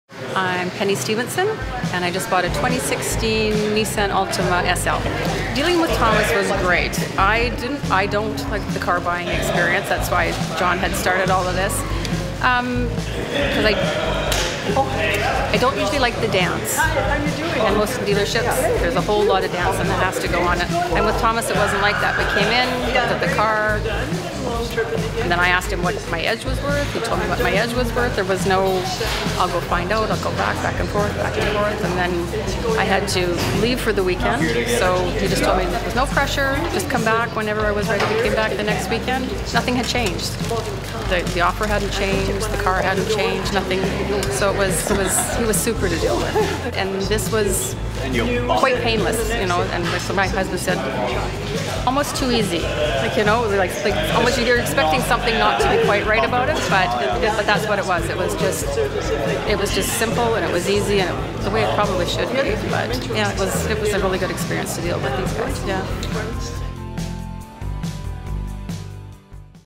Penny Had a Great Experience With Thomas | West Coast Nissan
Penny Stephenson purchased a 2016 Nissan Altima SL from West Coast Nissan. She does not like the process of purchasing a vehicle, but she found with Thomas there was no “dancing”. Thomas was able to provide Penny with the information she requested with no pressure to trade-in or purchase, she came back the next weekend and purchased.

“Nothing had changed, the offer hadn’t changed, the car hadn’t changed, nothing!” - Penny

Congratulation Penny on your 2016 Nissan Altima SL.

Transcript Below:

I’m Penny Stephenson, and I just bought a 2016 Nissan Altima SL.

Dealing with Thomas was great! I don’t like the car buying experience that’s why John had started all of this. I don’t usually like the dance. At most dealerships, there’s a whole lot of dancing that has to go on. With Thomas, it wasn’t like that.

We came in, looked at cars and I asked him what my Edge was worth. He told me what my Edge was worth; there was no I’ll go find out, go back and forth, back and forth. I had to leave for the weekend, so he just told me there was no pressure just come back whenever I was ready, and I came back the next weekend.

Nothing had changed, the offer hadn’t changed, the car hasn't changed,

He was super diligent, and this was quite painless, and as my husband said, almost too easy, you know. You’re expecting something not quite right about it, but that’s what it was. It was just simple; it was easy and the way it probably should be. Simply, a really good experience to deal with these guys.

West Coast Nissan
19625 Lougheed Hwy
Pitt Meadows, BC
V3Y 1Z2
Canada